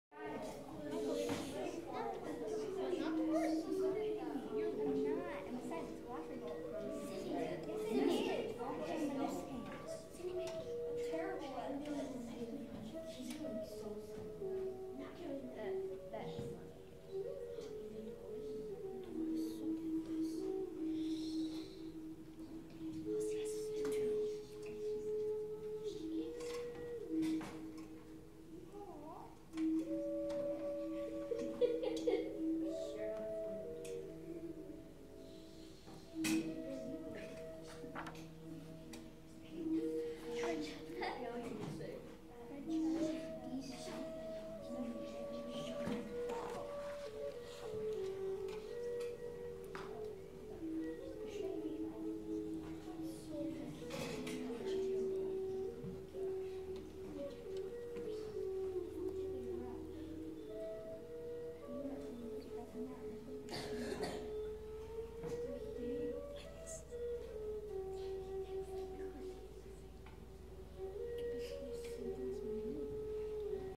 Japanese Kanji 2017
Woodland World Studies students try writing Japanese Calligraphy with traditional bamboo brushes. Spring semester 2017